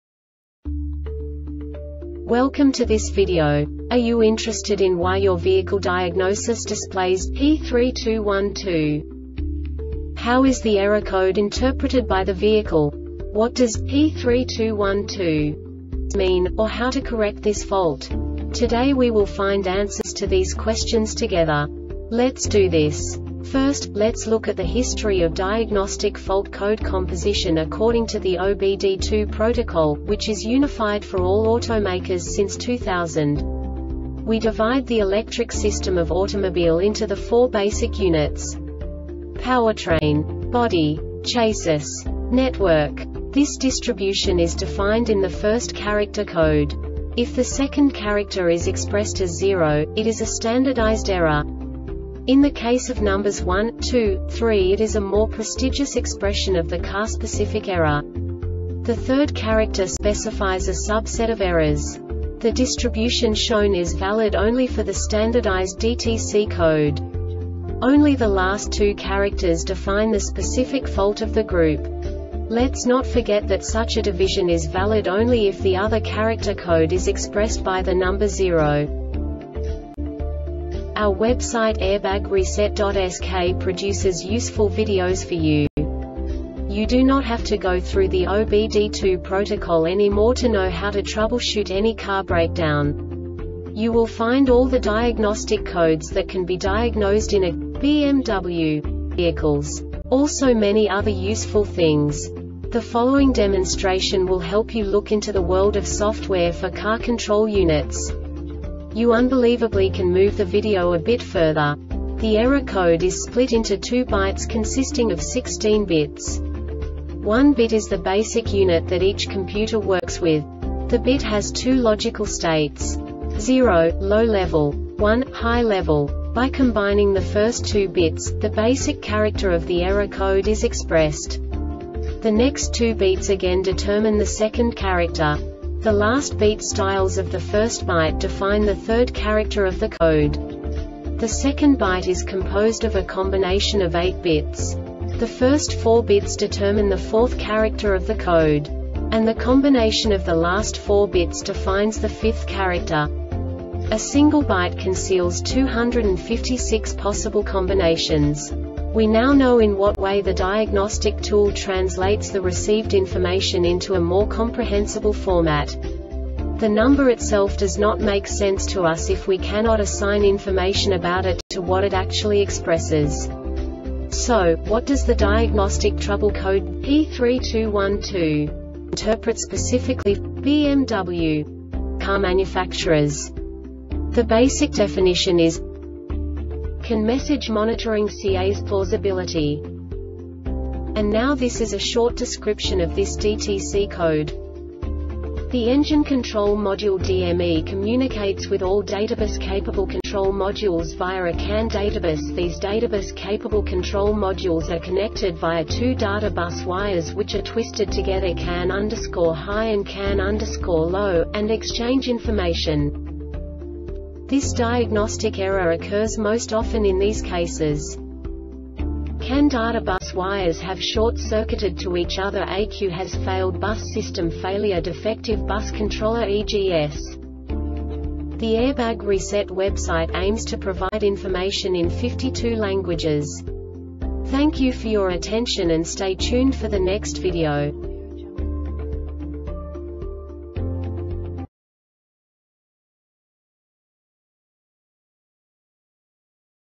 DTC BMW P3212 Short Explanation
Contents:
0:21 Basic DTC analysis according to OBD2 protocol standard.
1:48 Insight into programming
2:58 A diagnostically understandable description of the Diagnostic Trouble Code
3:05 Basic error definition

Make:
BMW
Code:
P3212
Definition:
CAN Message Monitoring CAS Plausibility
Description:
The Engine Control Module (DME) communicates with all databus-capable control modules via a CAN databus. These databus-capable control modules are connected via two data bus wires which are twisted together (CAN_High and CAN_Low), and exchange information
Cause:
CAN data bus wires have short circuited to each other
ECU has failed
BUS system failure
Defective bus controller (EGS)